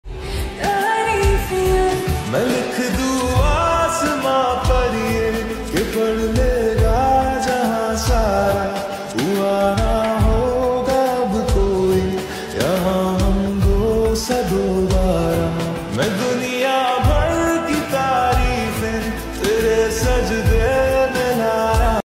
MARYSE OUELLET ROMANTIC HINDI SONG
I LOVE MARYSE SO I EDIT MARYSE VDEO IN HINDI SONG